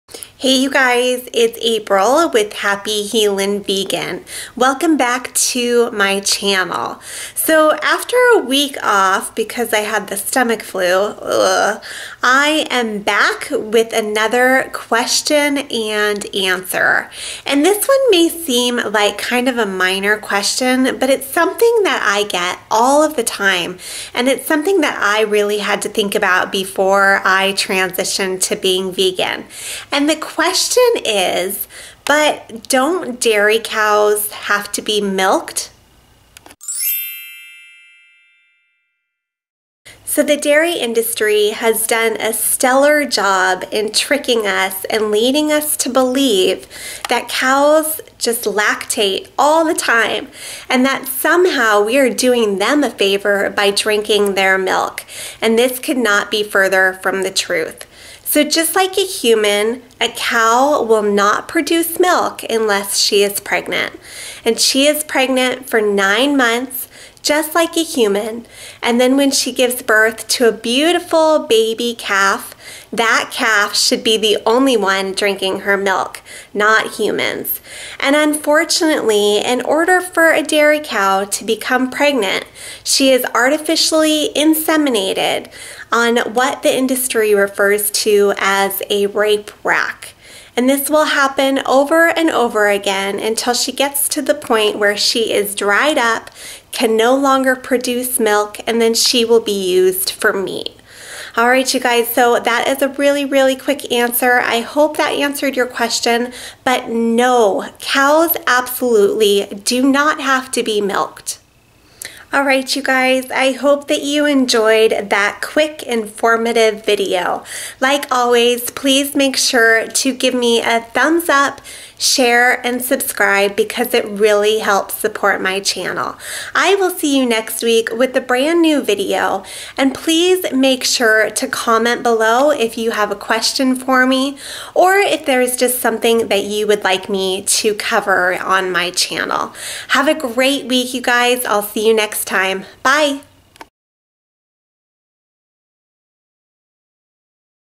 Don't Cows Have to Be Milked?
So after a week off, because of getting hit with the stomach flu, I am back with another question and answer.  This may seem like it's not very important, but as a vegan I get asked this question all of the time:

Because the diary industry has done a stellar job through their marketing, my answer may surprise you a bit.

I hope that you will check out my quick, informative answer to find out for yourself if cows need to be milked.  Are we doing them a favor?

*All images were found on Google.